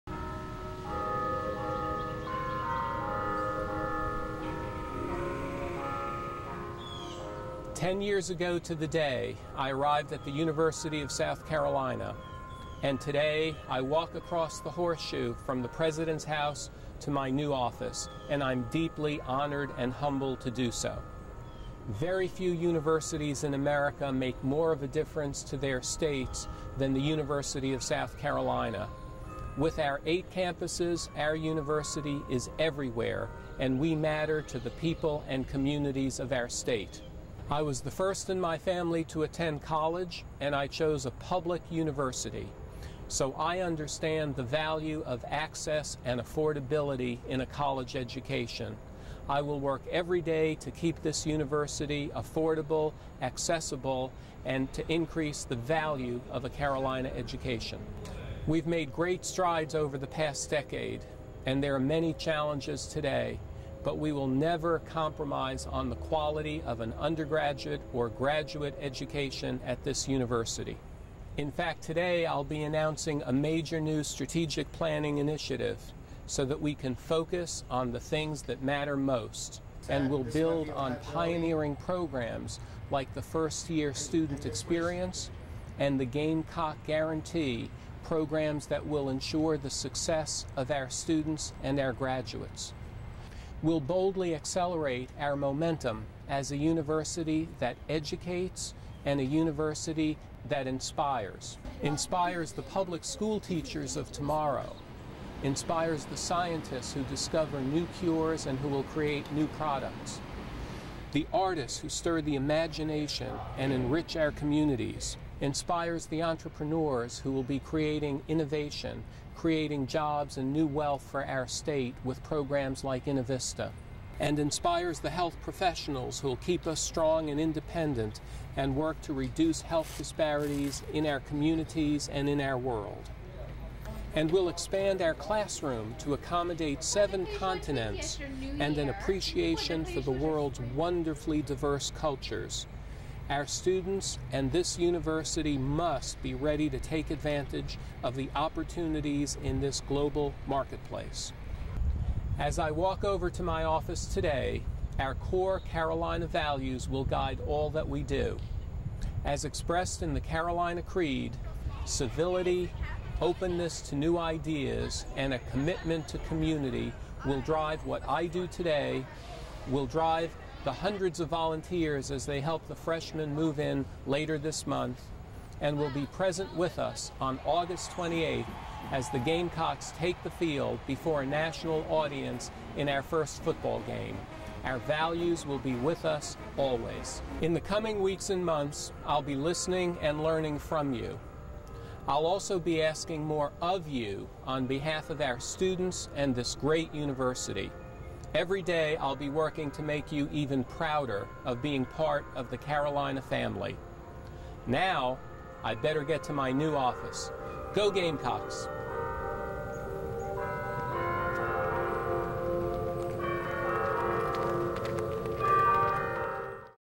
First Day
August 1, 2008- Harris Pastides' first day as president of the University of South Carolina.